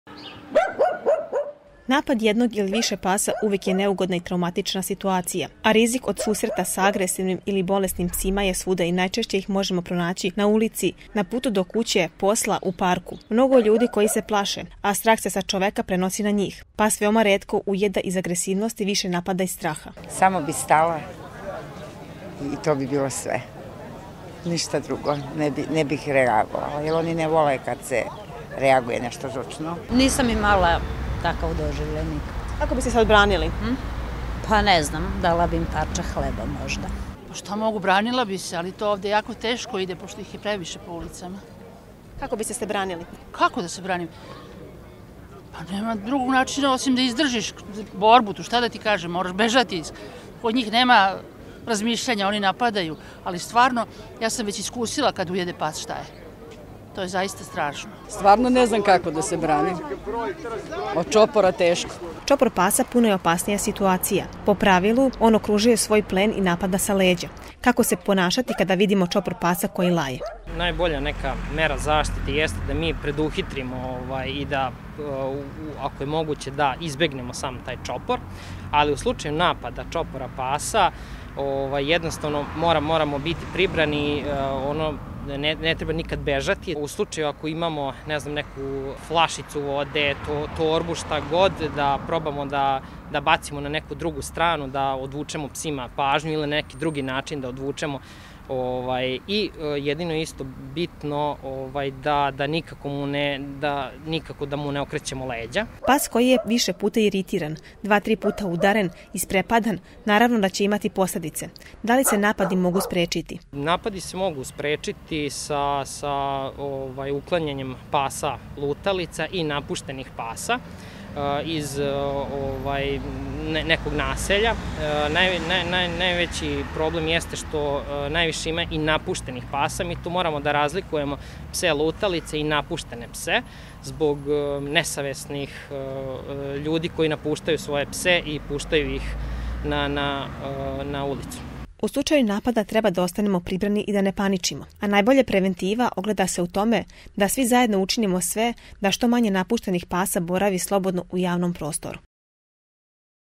Kako izbeći napad pasa lutalica
Veliki je hendikep kad zbog straha ne možemo da pomazimo ulične pse, koji bi sve dali da budu udimljeni. Čopor pasa nije uvek opasnost, čak i kad laju ne znače da im je namera da nas ujedu, uglavnom poručuju da im smetamo i da treba da se sklonimo, ali šta raditi ukoliko krenu u napad?